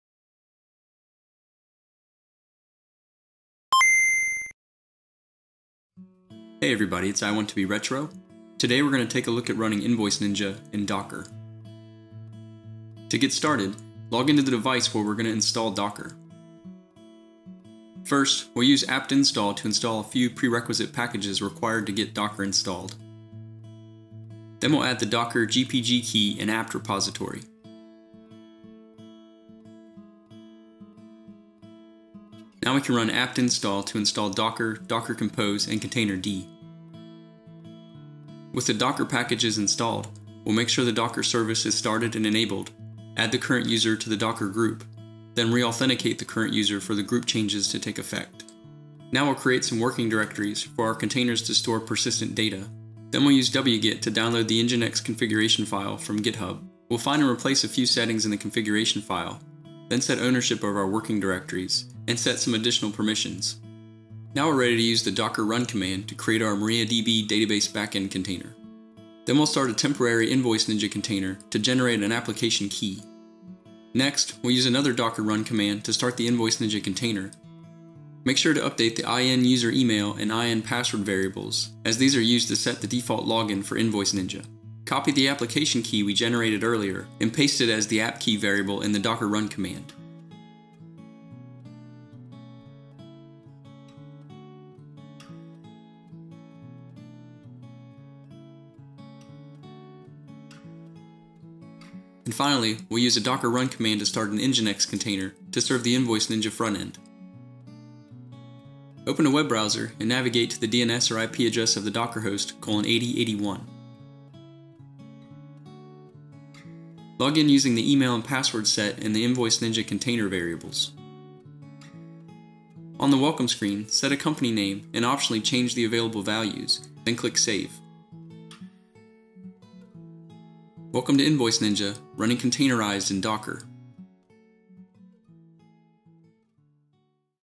Run InvoiceNinja - Invoice and Payment Manager - in Docker
*** Updated 05/24/2024

What is InvoiceNinja?
[InvoiceNinja is a] powerful suite of features to invoice clients, facilitate payment, track-time & tasks, expenses, and more.

Installing Docker
   01. Log into the Linux based device
   02. Run the following commands in the terminal
         # install prerequisites
         sudo apt install apt-transport-https ca-certificates curl software-properties-common gnupg-agent -y
         # add docker gpg key
curl -fsSL -F'=' '/^ID=/{ print $NF }' /etc/os-release)/gpg | sudo apt-key add
         # add docker software repository
sudo add-apt-repository "deb [arch=$(dpkg --print-architecture)] -F'=' '/^ID=/{ print $NF }' /etc/os-release) $(lsb_release -cs) stable"
         # install docker
         sudo apt install docker-ce docker-compose containerd.io -y
         # enable and start docker service
         sudo systemctl enable docker && sudo systemctl start docker
         # add the current user to the docker group
         # reauthenticate for the new group membership to take effect
         su - $USER

Running the InvoiceNinja Container
   01. Now that Docker is installed, run the following commands to setup the InvoiceNinja Docker container and run it
         # create working directories
         mkdir ~/docker/invoiceninja/{public,storage} -p && mkdir ~/docker/mariadb -p && mkdir ~/docker/nginx -p
         # update the config file with the invoiceninja container name
         sed -i 's/fastcgi_pass app:9000;/fastcgi_pass invoiceninja:9000;/g' ~/docker/nginx/invoiceninja.conf && sed -i 's/listen 80 default_server;/listen 80;/g' ~/docker/nginx/invoiceninja.conf
         # set ownership on the working directories
         sudo chown "$USER":"$USER" ~/docker -R
         # allow the container to write to files/ and settings.php
         sudo chmod a+rwx -R ~/docker/invoiceninja
         # run the mariadb docker container
         docker run -d --name mariadb -e MYSQL_ROOT_PASSWORD=r00tp@$$ -e MYSQL_USER=invoiceninja_rw -e MYSQL_PASSWORD='!nv0ic3Ninj@!' -e MYSQL_DATABASE=invoiceninja -v ~/docker/mariadb:/var/lib/mysql -p 3306:3006 --restart=unless-stopped mariadb:latest
         # generate application key
         docker run --rm -it invoiceninja/invoiceninja php artisan key:generate --show
         # run the invoiceninja container
         # change the IN_USER_EMAIL and IN_PASSWORD variables to setup the default user
         # update the APP_KEY variable with the key generated earlier
-e REQUIRE_HTTPS=false -e APP_ENV='production' -e APP_DEBUG=0 -e APP_URL=' -e APP_KEY='≪%key generated earlier%≫' -e APP_CIPHER='AES-256-CBC' -e DB_TYPE='mysql' -e DB_STRICT='false' -e DB_HOST='mariadb' -e DB_DATABASE='invoiceninja' -e DB_USERNAME='invoiceninja_rw' -e DB_PASSWORD='!nv0ic3Ninj@!' --restart=unless-stopped invoiceninja/invoiceninja
         # run the nginx container
         docker run -d --name nginx --link invoiceninja -p 8081:80 -v ~/docker/invoiceninja/public:/var/www/app/public:ro -v ~/docker/nginx:/etc/nginx/conf.d:ro --restart=unless-stopped nginx
02.
   03. Login with the Email and Password setup in the InvoiceNinja container variables ≫ Click Login with email
   04. On the Welcome dialog, enter a company name and select the Light or Dark theme ≫ Click Save
   05. Welcome to InvoiceNinja